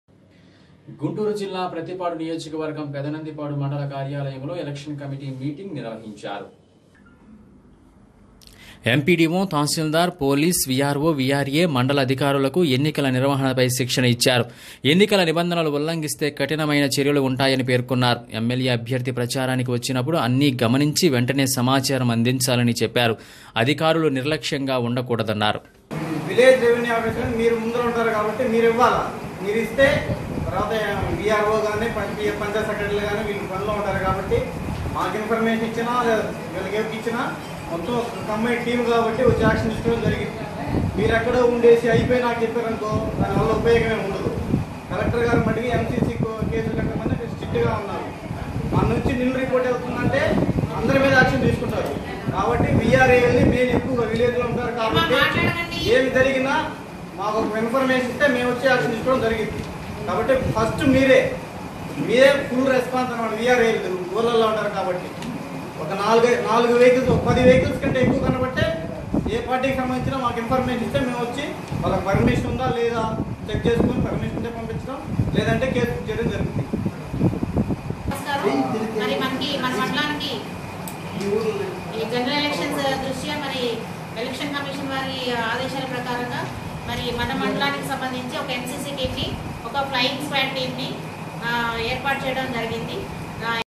Review of election management with election staff in prathipadu || Dn5
గుంటూరు జిల్లా ప్రత్తిపాడు నియోజకవర్గం పెదనందిపాడు మండల కార్యాలయంలో ఎలక్షన్ కమిటీ మీటింగ్ నిర్వహించారు.
ఎంపీడీవో, తహశీల్దారు, పోలీస్, వీఆర్వో, వీఆర్ఏ, మండల అధికారులకు ఎన్నికల నిర్వహణపై శిక్షణ ఇచ్చారు. ఎన్నికల నిబంధనలు ఉల్లంగిస్తే కఠినమైన చర్యలు ఉంటాయని పేర్కొన్నారు. ఎంఎల్ఎ అభ్యర్థి ప్రచారానికి వచ్చినప్పుడు అన్ని గమనించి వెంటనే సమాచారం అందించాలని చెప్పారు. అధికారులు నిర్లక్ష్యంగా  ఉండకూడదన్నారు.